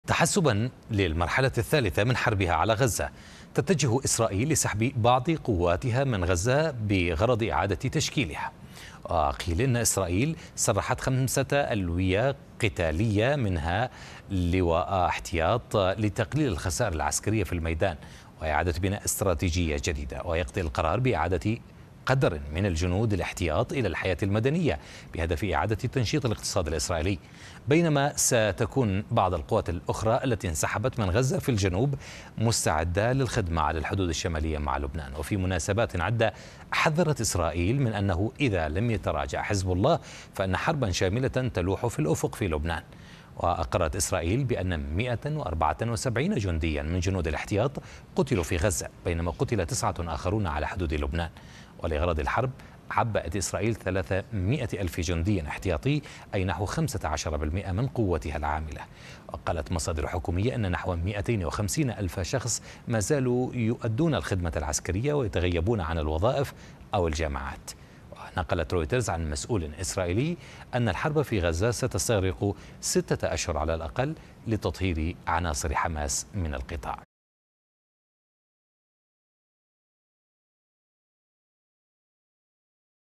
إسرائيل تُسرح 5 ألوية قتالية من غزة
بغرض تشكيلها.. إسرائيل تُسرح 5 ألوية قتالية من قطاع غزة لتقليل الخسائر العسكرية في الميدان وإعادة بناء استراتيجية جديدة